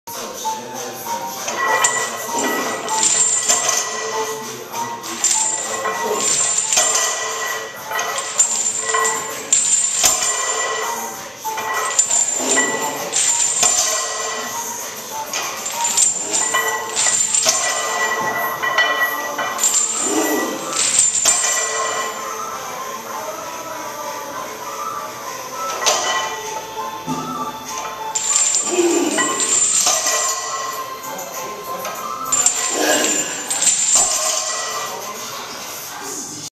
McConnell Athletics/ High-rep TBDL w/chains
Trap Bar DeadLifts w/'heavy' chains added. Application for hip extension/ strength.